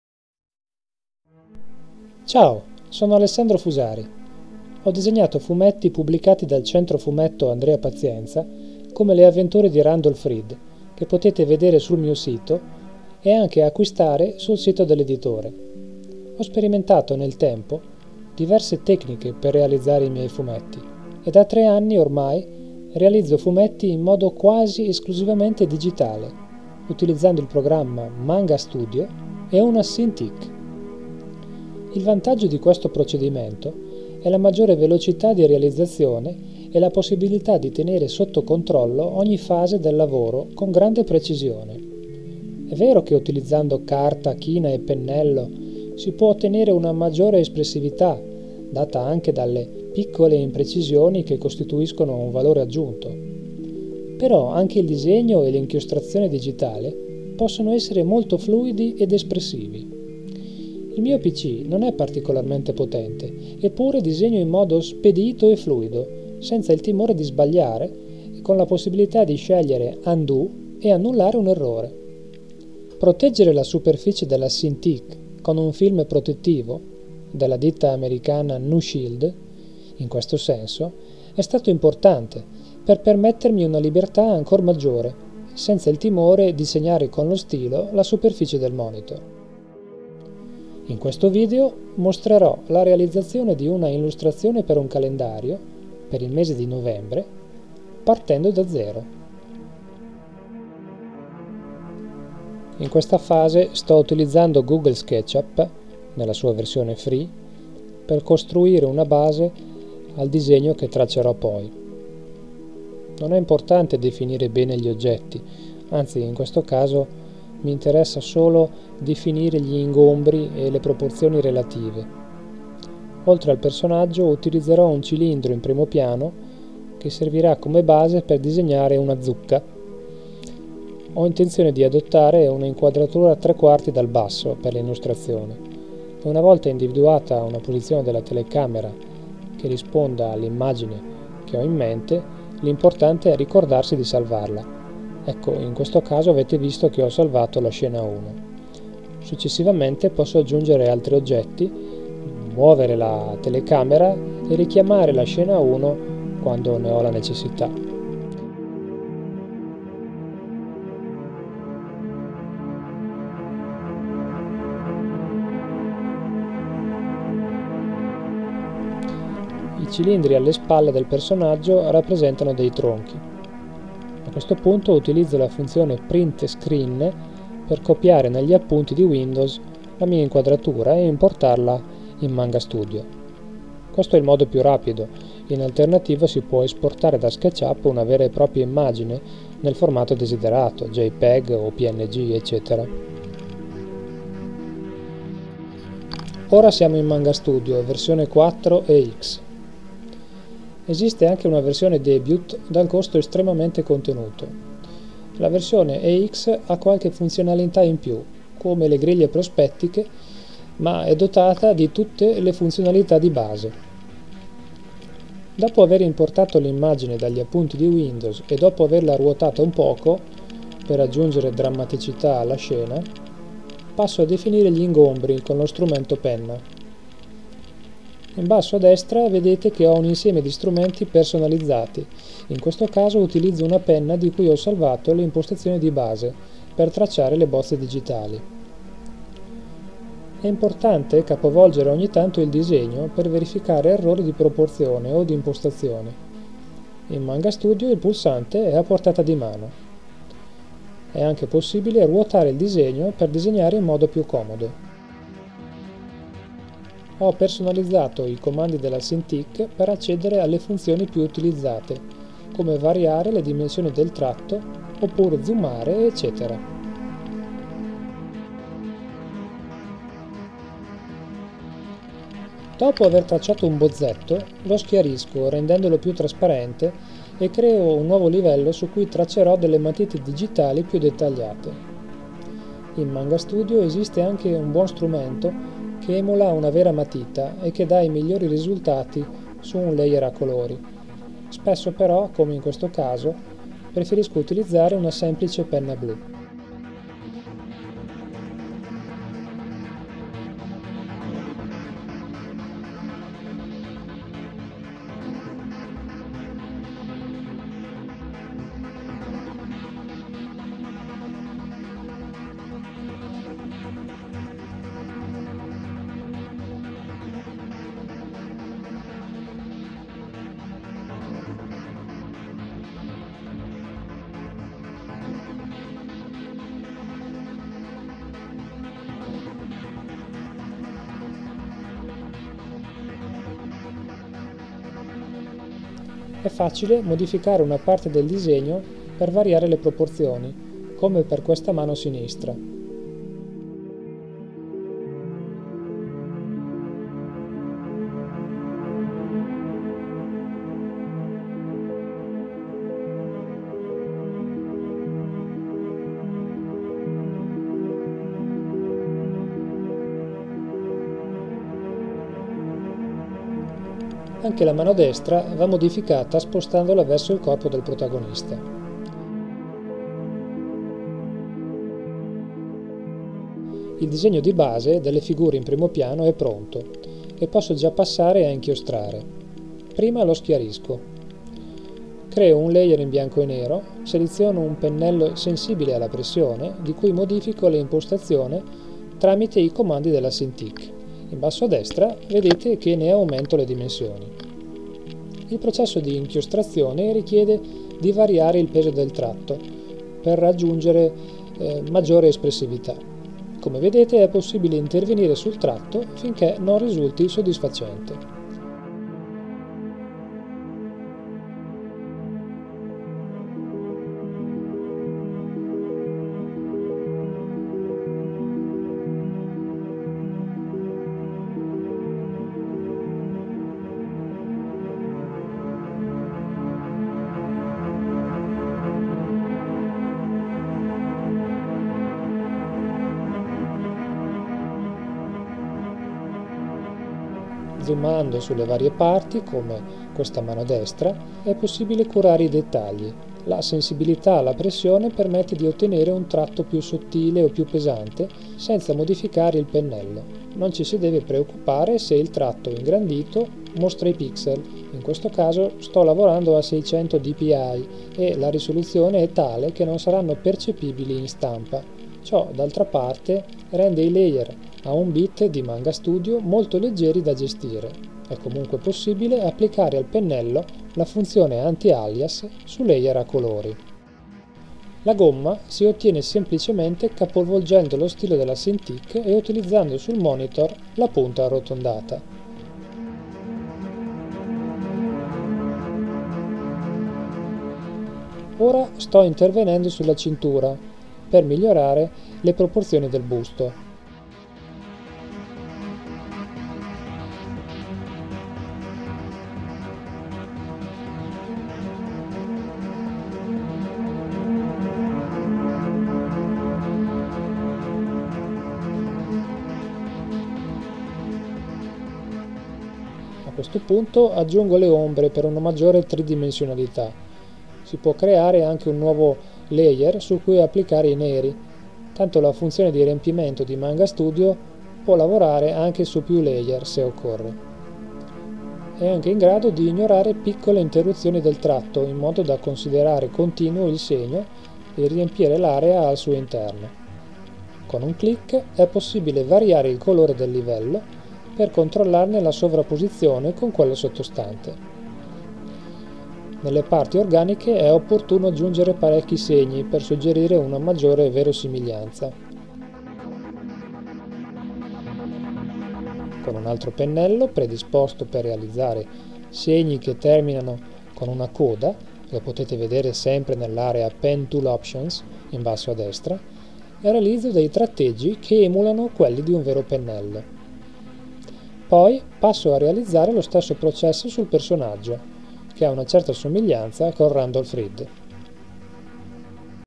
Illustrazione digitale - Parte 1/2
Realizzazione di una illustrazione digitale con Manga Studio EX 4 e una Cintiq 21UX: bozza, disegno, inchiostrazione e colorazione. Parte 1 di 2.